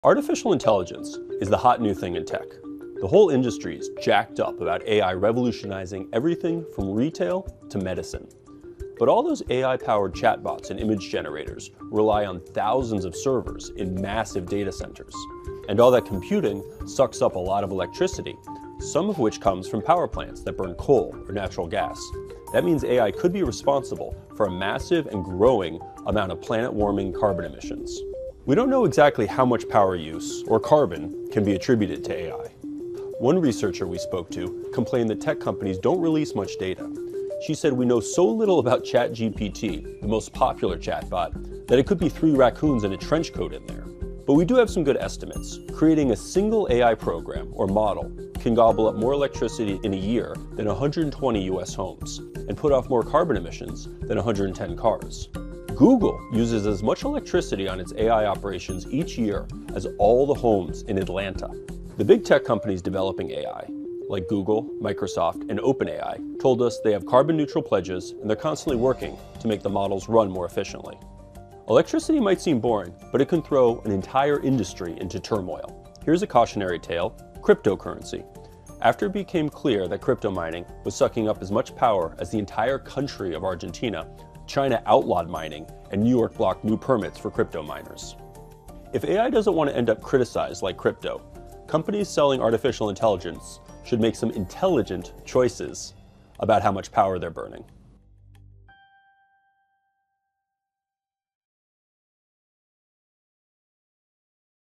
What the AI Boom Means for Carbon Emissions
Artificial Intelligence is booming, and so is its carbon footprint. AI is the tech industry's new toy, with expectations it’ll revolutionize trillion-dollar industries from retail to medicine. But the creation of every new chatbot and image generator requires a lot of electricity, which means the technology may be responsible for a massive and growing amount of planet-warming carbon emissions.